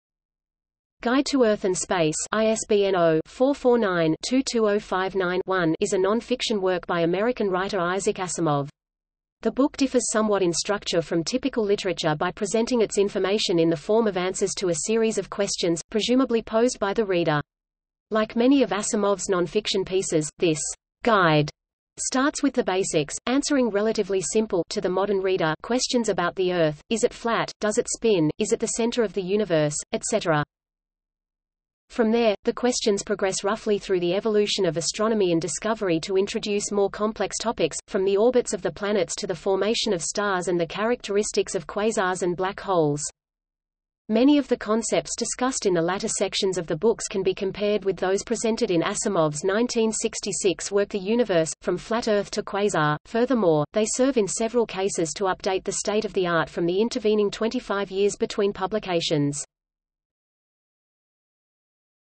Isaac Asimov's Guide to Earth and Space | Wikipedia audio article
- Socrates

SUMMARY
Guide to Earth and Space (ISBN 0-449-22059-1) is a non-fiction work by American writer Isaac Asimov. The book differs somewhat in structure from typical literature by presenting its information in the form of answers to a series of questions, presumably posed by the reader. Like many of Asimov's non-fiction pieces, this "Guide" starts with the basics, answering relatively simple (to the modern reader) questions about the Earth - is it flat, does it spin, is it the center of the universe, etc...
From there, the questions progress roughly through the evolution of astronomy and discovery to introduce more complex topics, from the orbits of the planets to the formation of stars and the characteristics of quasars and black holes.
Many of the concepts discussed in the latter sections of the books can be compared with those presented in Asimov's 1966 work The Universe: From Flat Earth to Quasar; furthermore, they serve in several cases to update the state of the art from the intervening 25 years between publications.